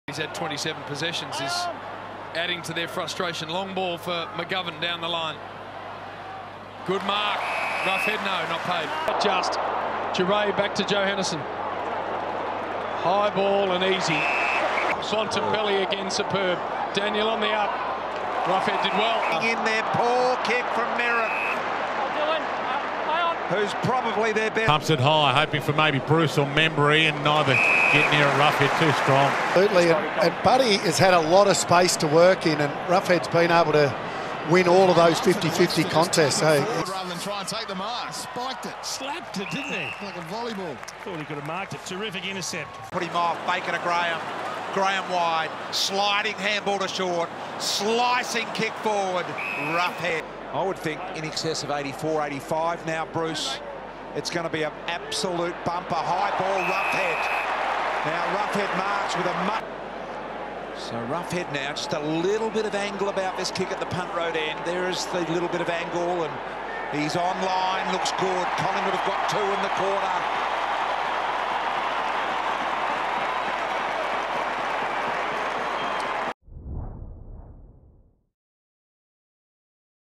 2019 Highlights: Jordan Roughead
Catch some of the best highlights of Jordan Roughead's 2019 season.